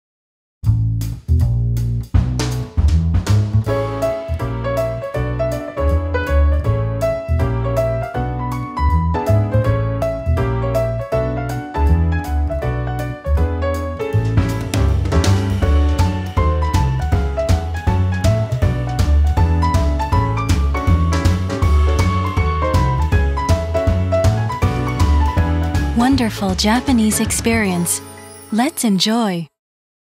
【PR Video】Japanese Food Supporter Store（ASIA Ver.）
PR movie of Japanese Food　Supporter Store.（ASIA Ver.）
JETRO certifies stores that are promoting Japanese-made foods abroad as a Japanese Food　Supporter Store.